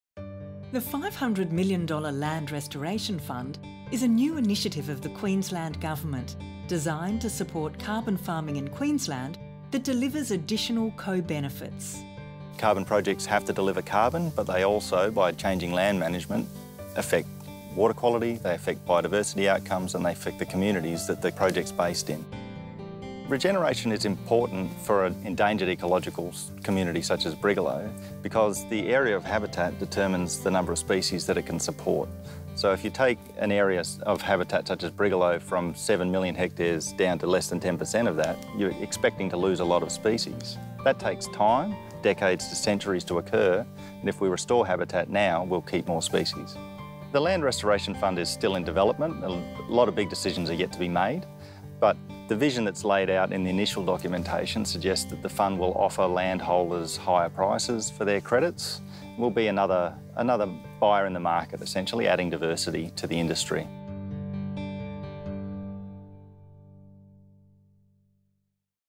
Land Restoration Fund - carbon farming
Queensland's carbon farming industry will find additional growth opportunities through the new $500 million Land Restoration Fund.